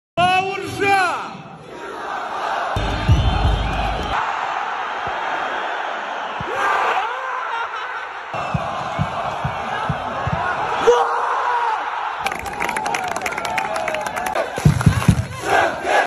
Ордабасы - Легия. Обзор матча. Лига Конференций
Ордабасы не только выбил Астану из Кубка страны и лидирует в чемпионате страны, команда из Шымкента дебютировала в Лиге Конференций УЕФА. Вашему вниманию ультракороткий обзор матча второго отборочного раунда Лиги Конференций УЕФА между Ордабасы Шымкент и Легия Варшава